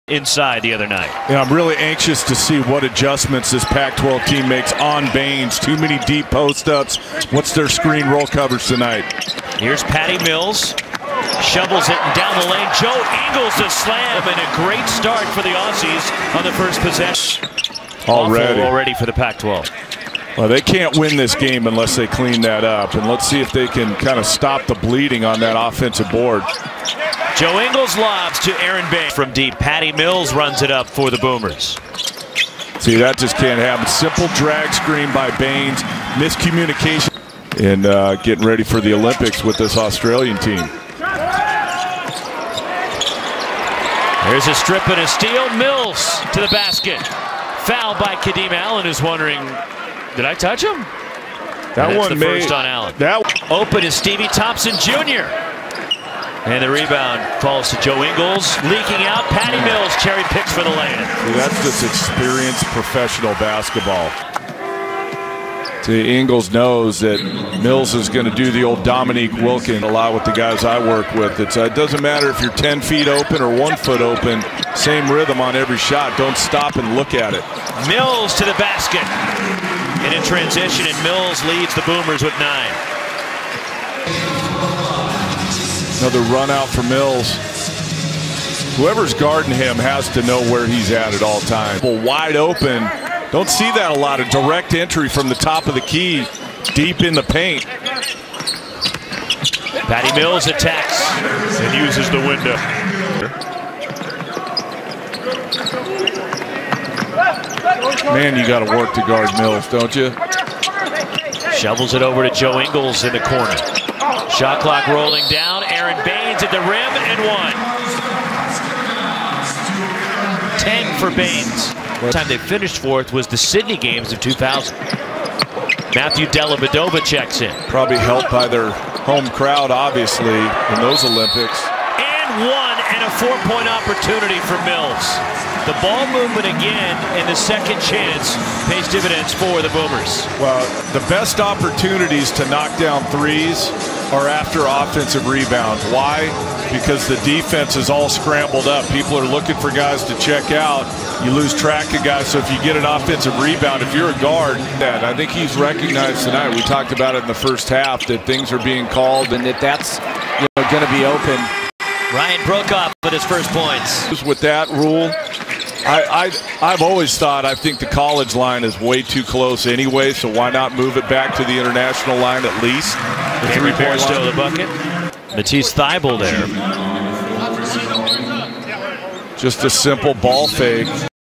Australia Boomers highlights | PAC-12 v Boomers Farewell Series G2 | 14 Jul 2016
The Boomers won game 2 of the Farewell Series in an easy 93-63 finish at Hisense Arena.

Joe Ingles (11 assists, 8 points), Patty Mills (29 points) and Baynes (19 points) led the team this night.

Highlights limited to first three quarters unfortunately - online stream failed to load in the final quarter.